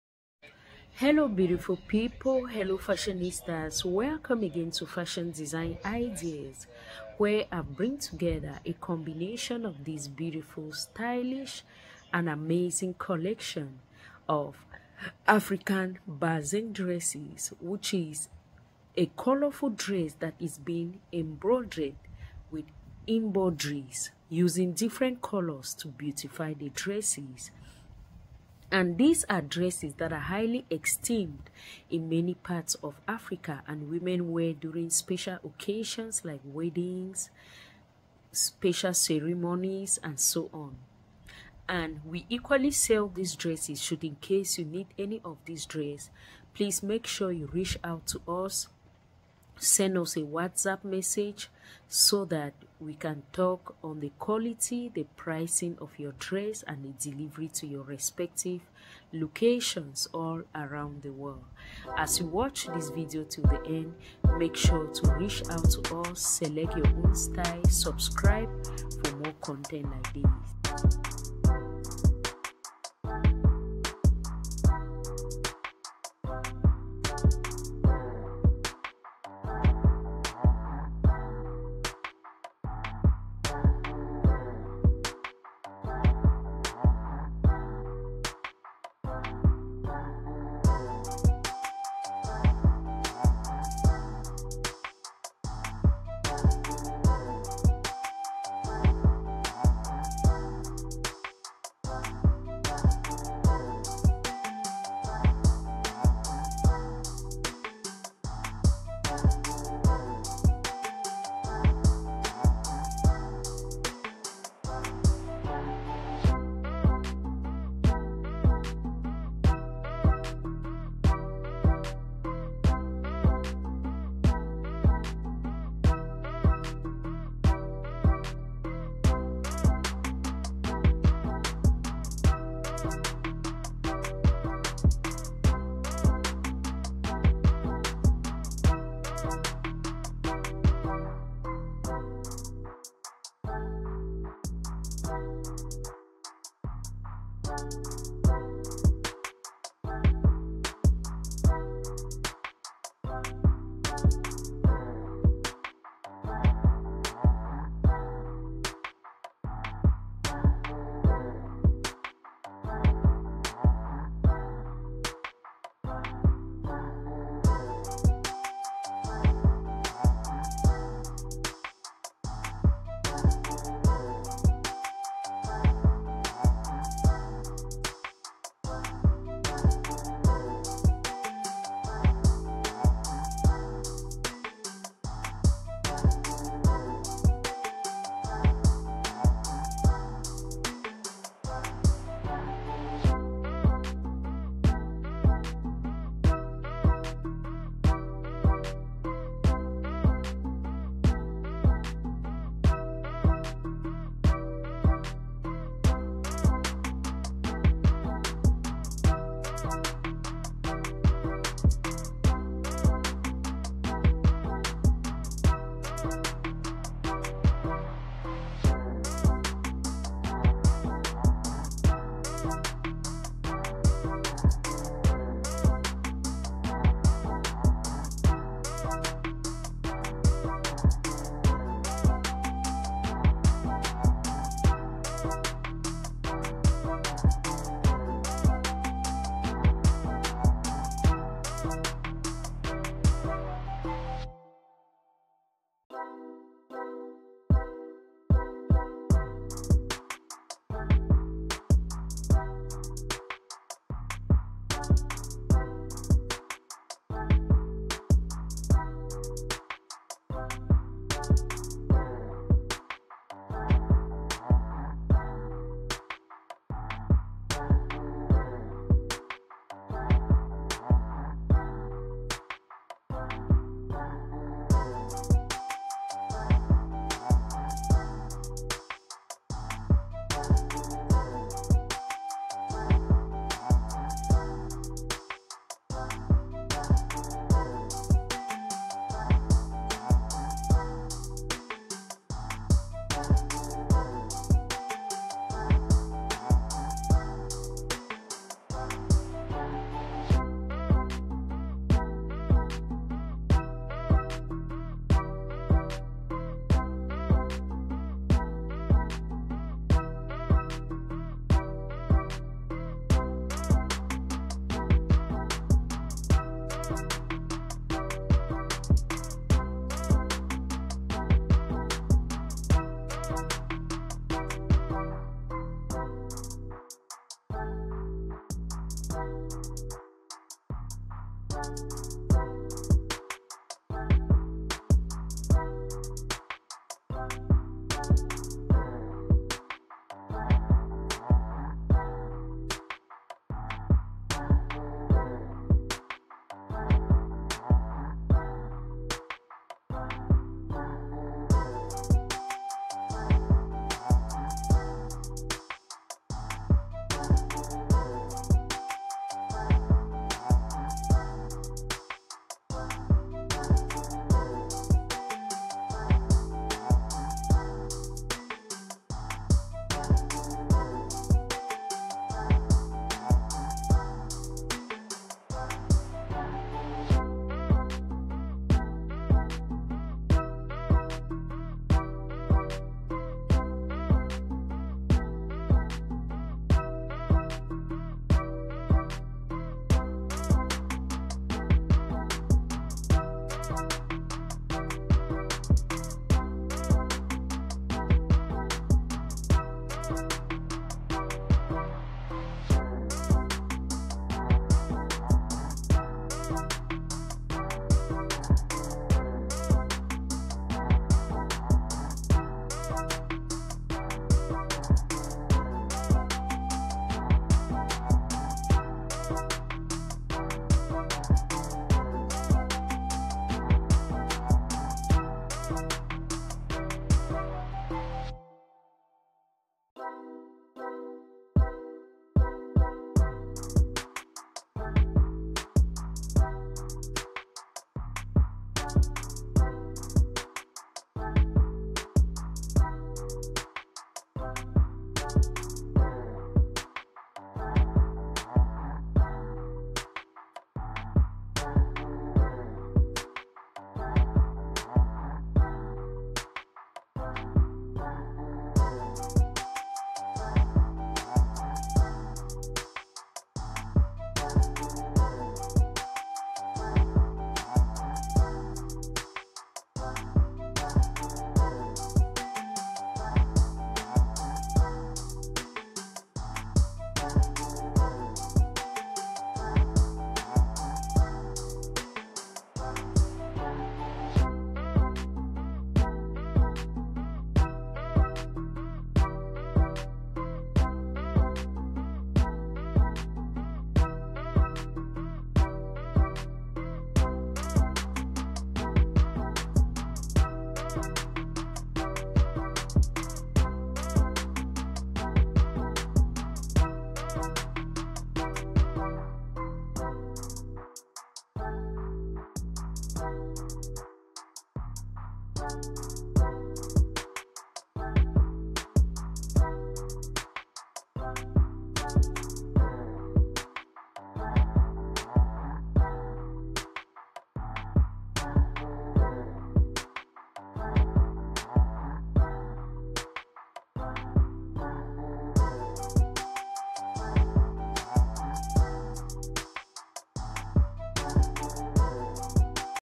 New collections of stylish bazin dress styles for beautiful ladies for beautiful women
Hello family welcome to fashionable long Ankara skirt and peplum blouse top design ideas. In this video i am bringing the latest African lace dresses to rock for weddings, special occasions and more which I have explain in details what all these dresses are all about.

In the video you will see different designs of Bazin Riche senegalesse styles of different styles, different colours.some of the dresses styles are high quality while others are low quality.

Furthermore, in the video you will see some of the styles  or almost all the styles that are very good for special occasions like weddings, special parties, also for church events as well as styles that you can wear to your shop, office and even casual.
As you watch make sure you select your own style for any purpose that you may like to wear or just get inspired by the different designs.